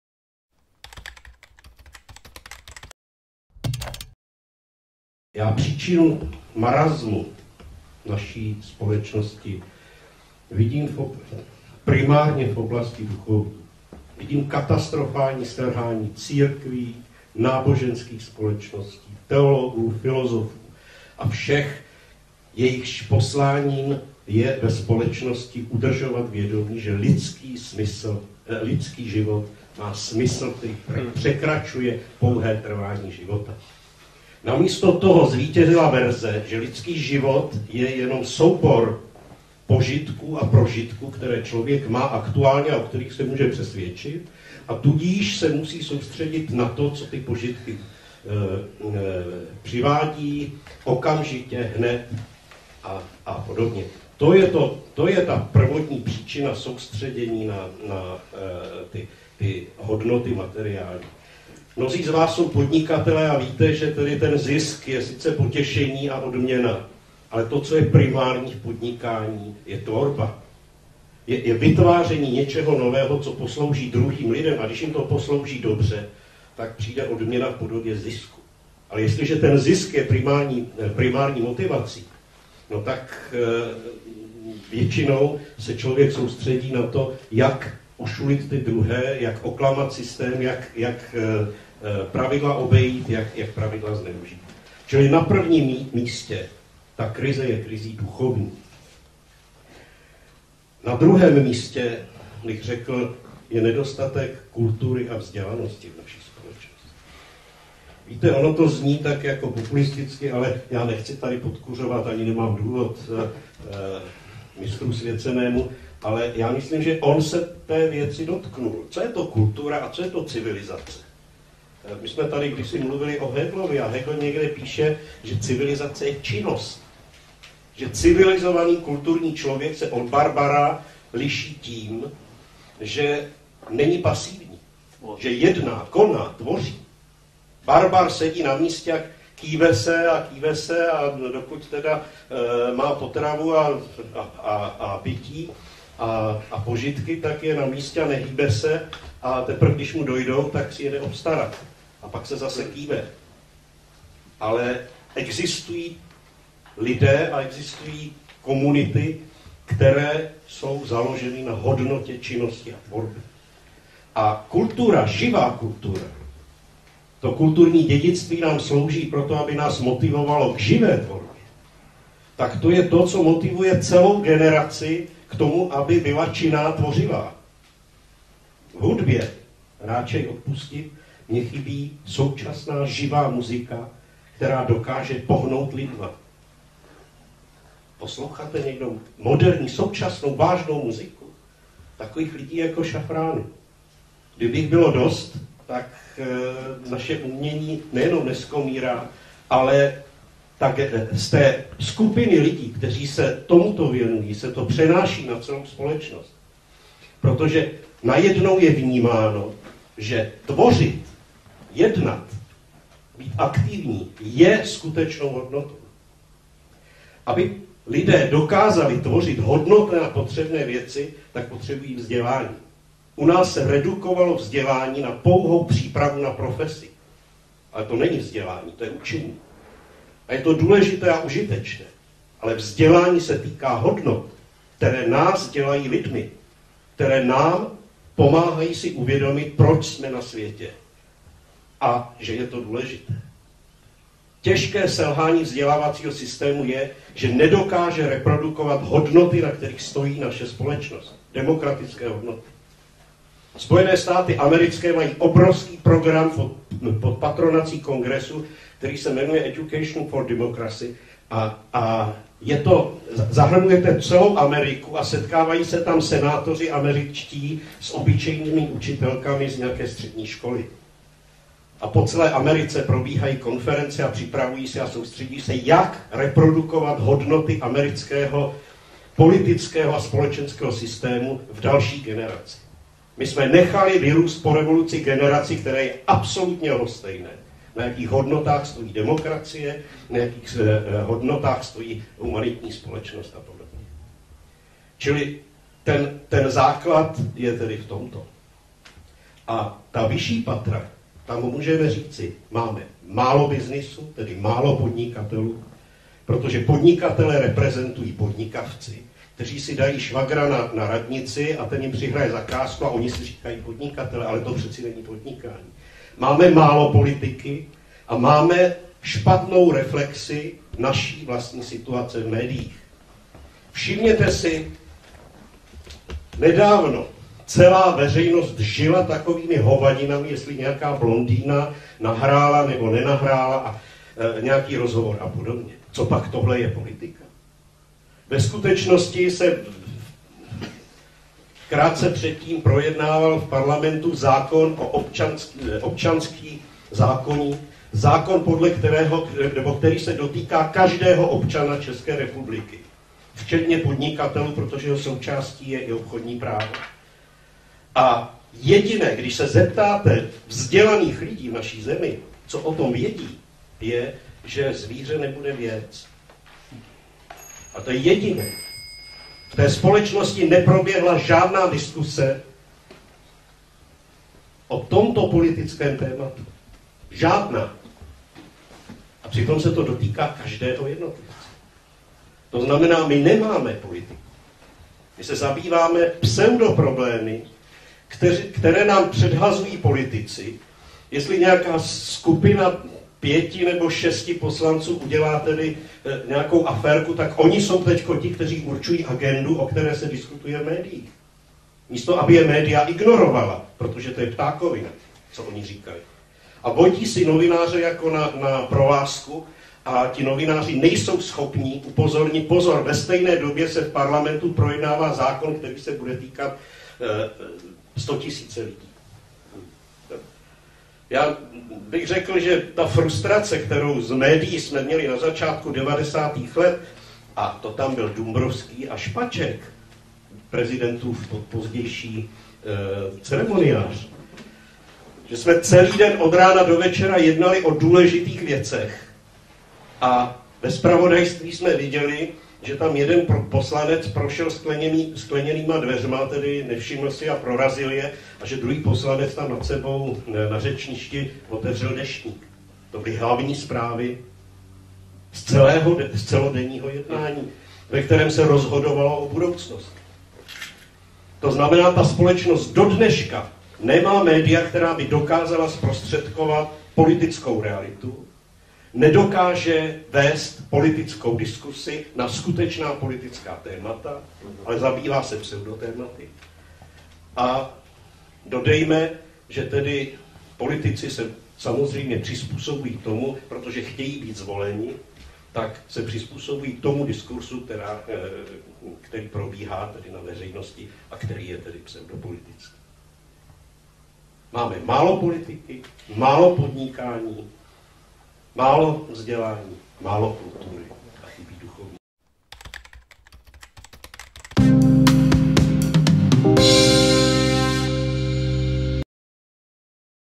Daniel Kroupa - Příčiny marasmu v naší společnosti
Krásná řeč filozofa a chartisty Daniela Kroupy o tom, kde on vidí příčiny problémů v naší zemi.